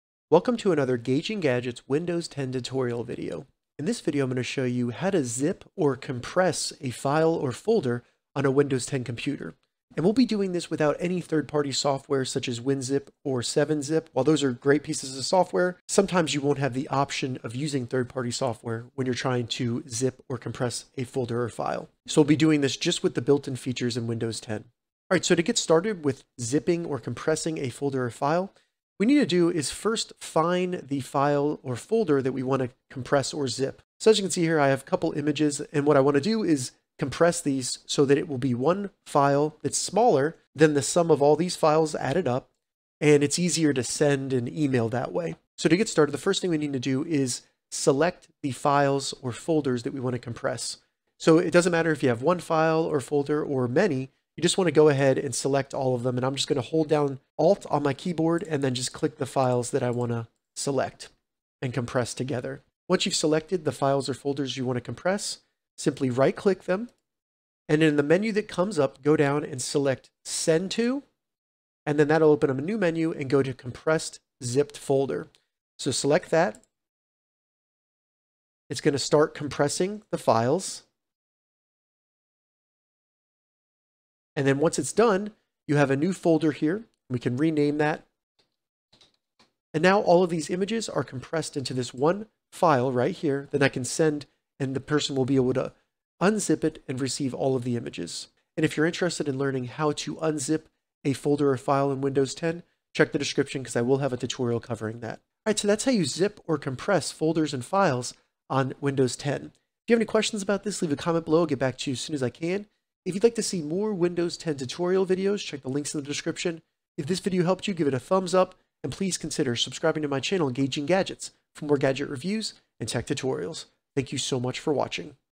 How To Zip a File Or Folder on Windows 10
How to zip a file or folder on Windows 10? In this tutorial, I show you how to zip or compress any file or folder on your Windows 10 PC. This means you can take many files and zip them into one file that is usually smaller in size.

Follow the steps below to zip files or folders on Windows 10:
1. Find and select the files on your Windows 10 computer that you'd like to zip or compress.
2. Right click the file.
3. In the menu, select Send to, then select Compressed (zipped) folder.
4. Name the new zipped folder.
5. You have now zipped a file or folder.